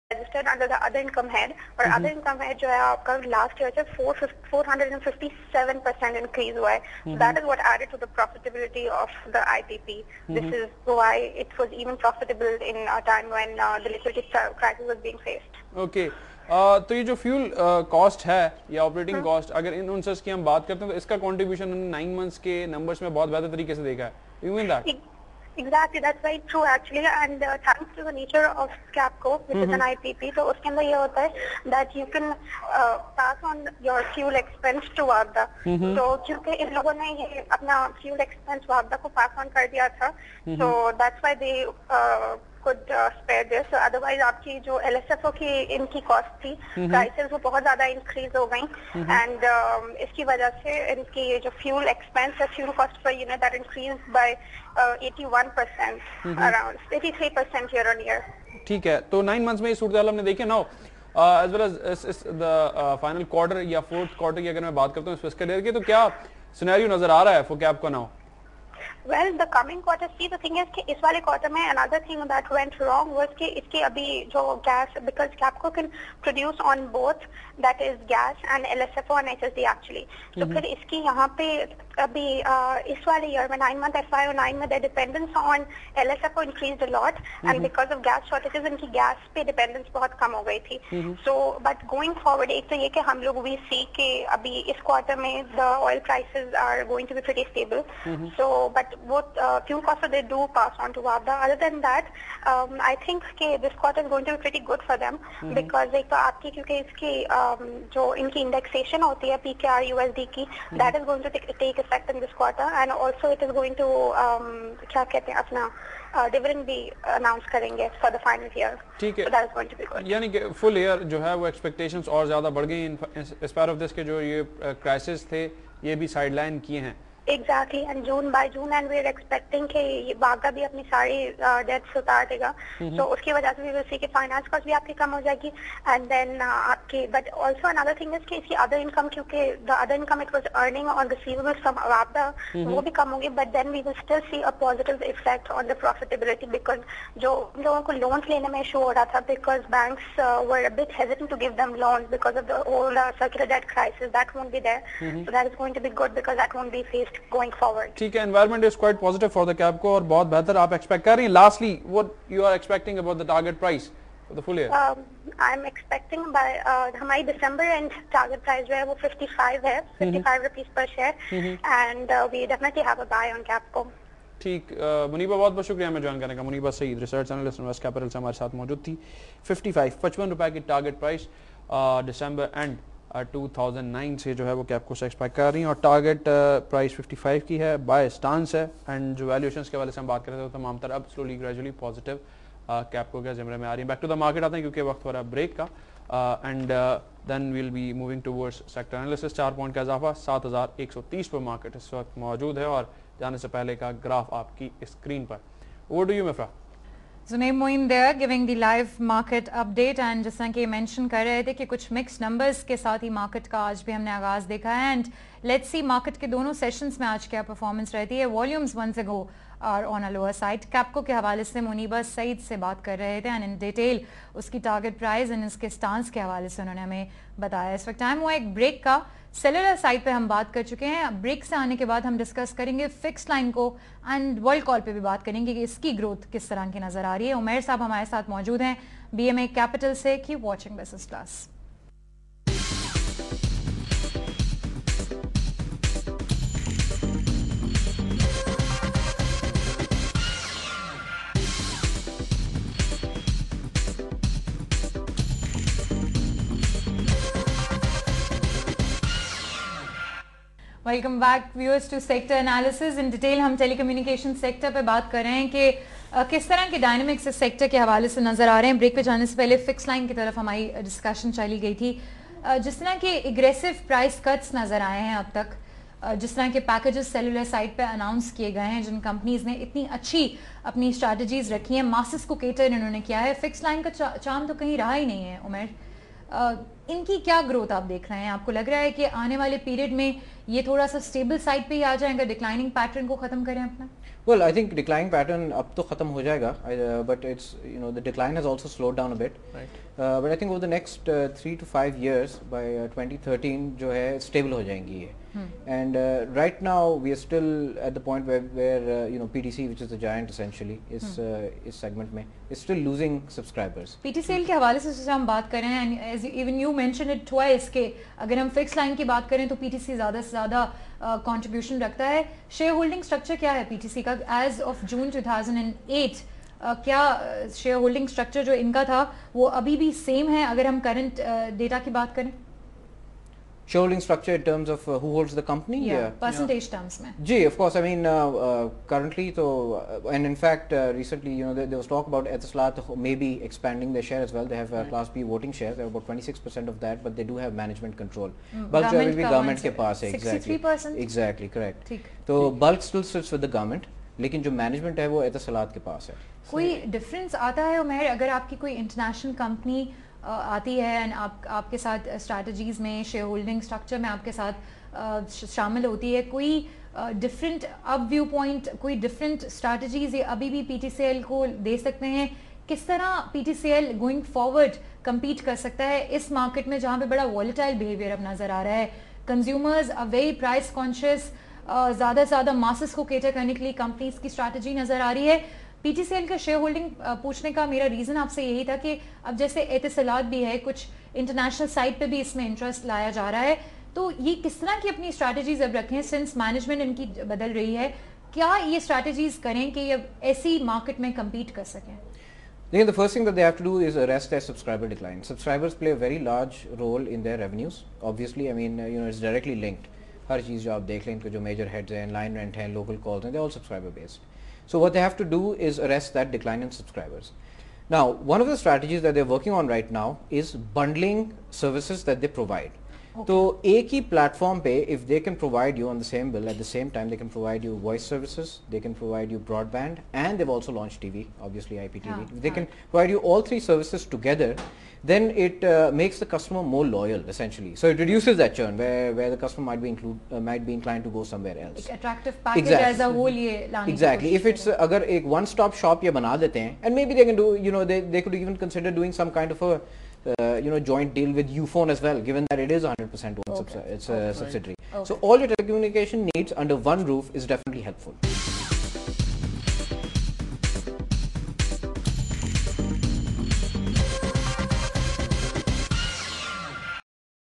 OmairChaghtai B+ Sector Analysis Part 04 (BMA Capital - May 2009)
BMA Capital Management Limited senior telecom analyst Omair Baig Chaghtai sector analysis program talks about the dynamics of telecom sector, its investment scenario and budget expectations for the sector for FY09 budget. He also talked about key economic challenges faced by telecom sector in Pakistan.